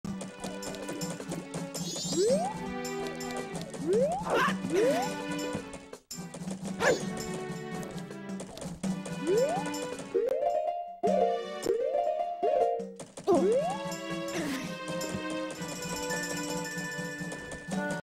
MM: Gainer
This video shows both the unbuffered and buffered way of doing a gainer.